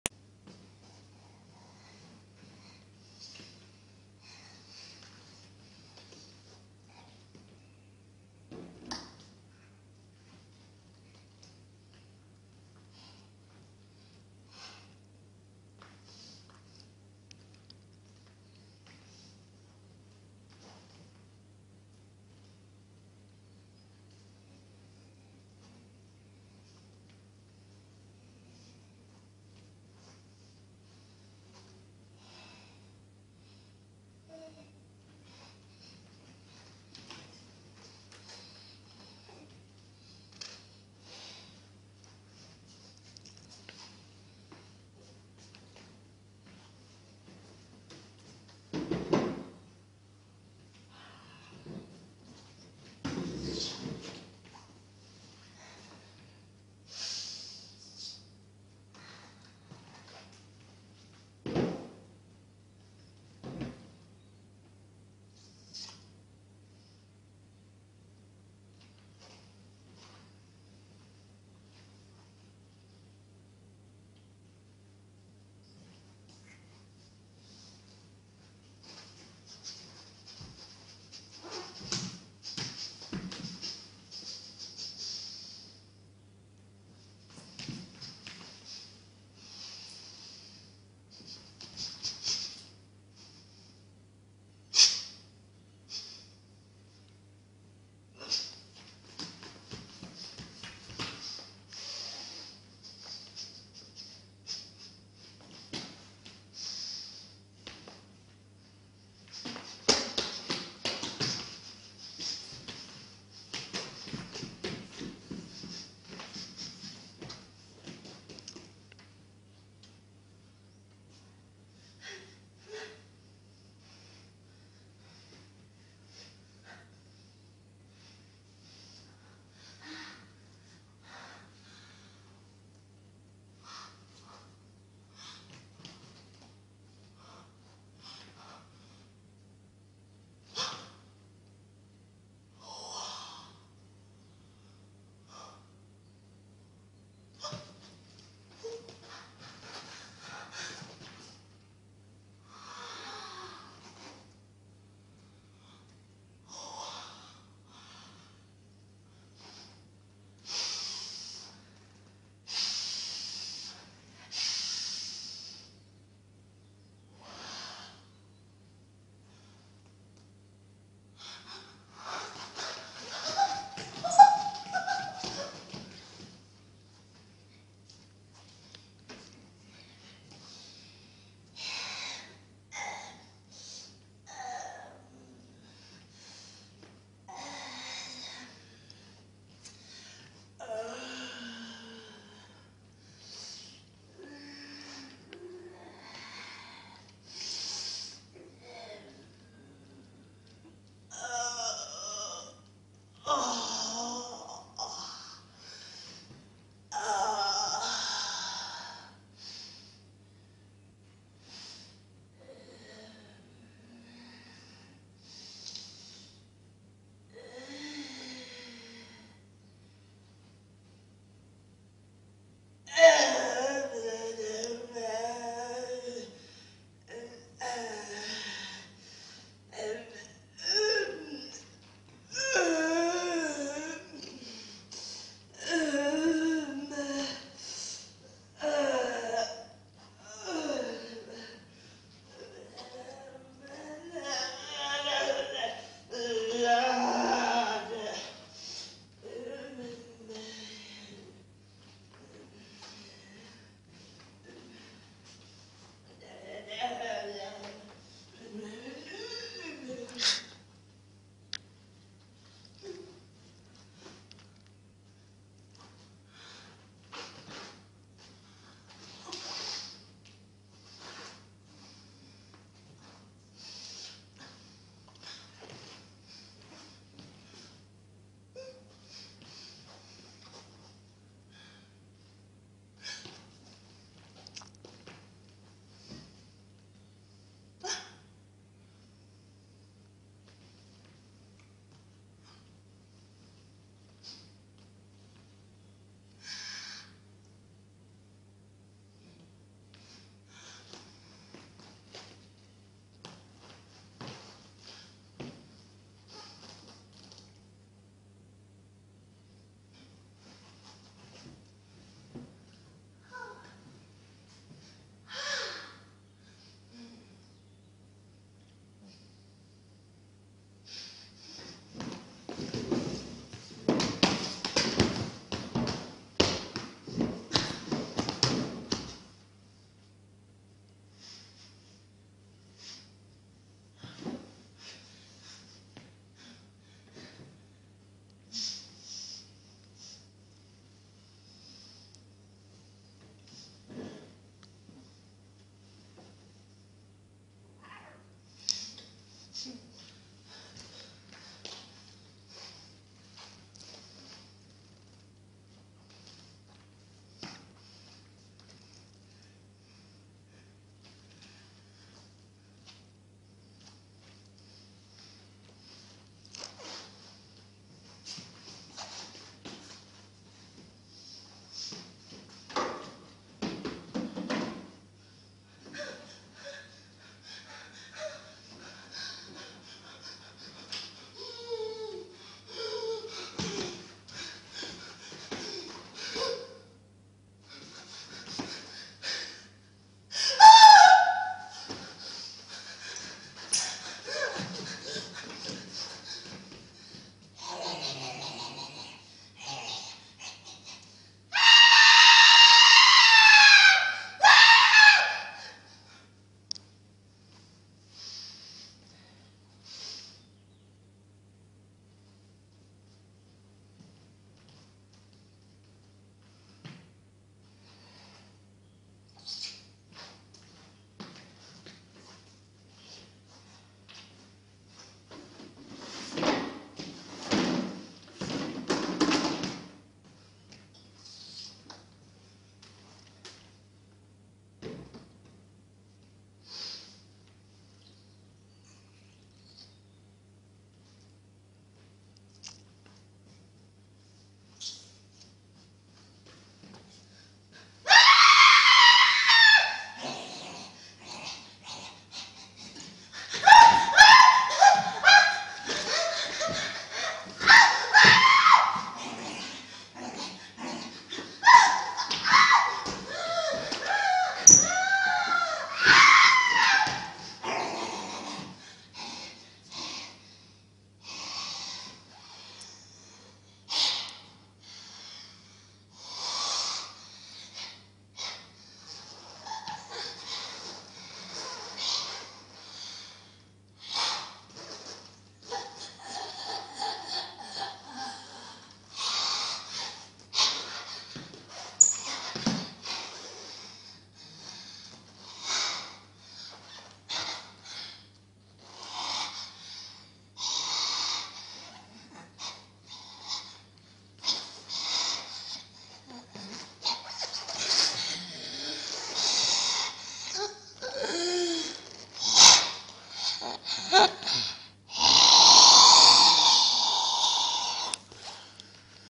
8 characters in 8 minutes
Improvised physical theatre piece, originally made for Cirque de Soleil audition. Creation and performance: Eden Avital Alexander. @EAlexande